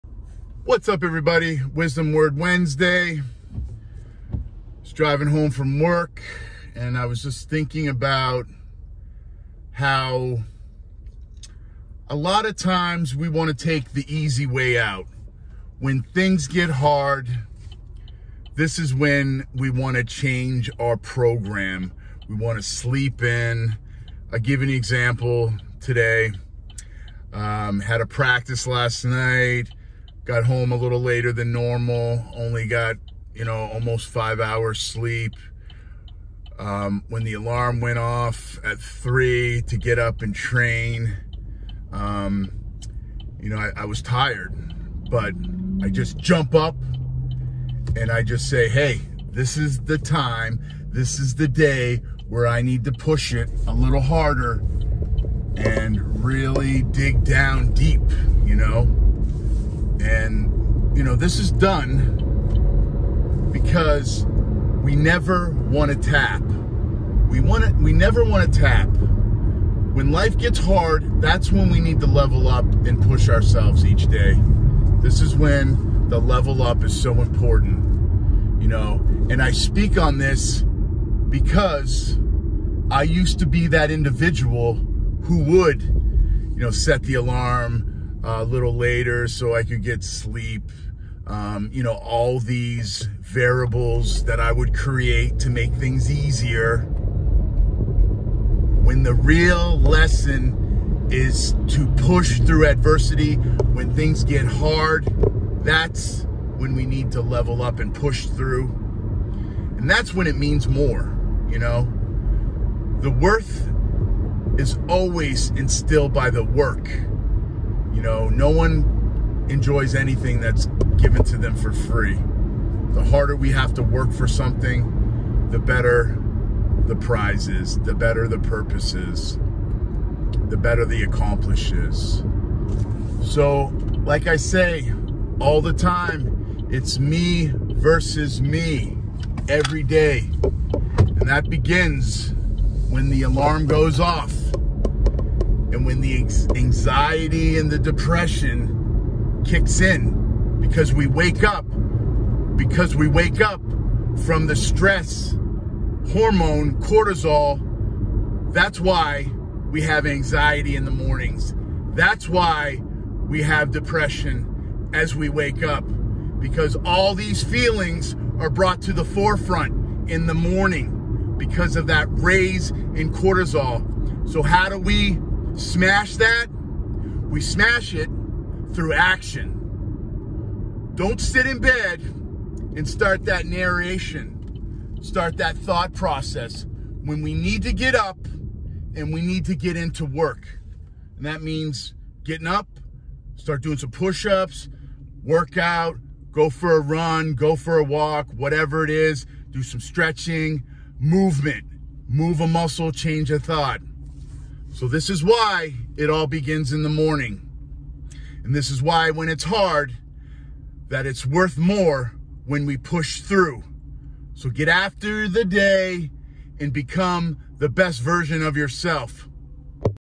Wisdom word Wednesday leveling up
When your faced with challenges don’t tap out and make the process easy, push through those days that are tough. Face adversity like a savage be that man that doesn’t break and smashes through each day like a champion.

Strive to be the best version of yourself daily.
Check out the bearded dude podcast on all podcast platforms.